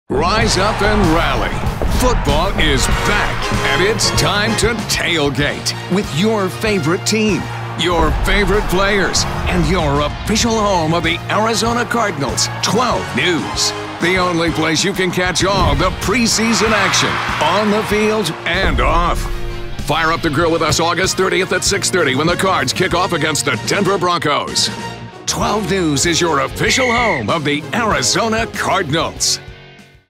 Football is Back! Cardinals Tailgate Thursday
It's time to tailgate with the Cardinal for a kick off against the Denver Broncos. 12 News is the official home of the Arizona Cardinals!
"12 News is Everything Arizona, Everywhere from A to Z"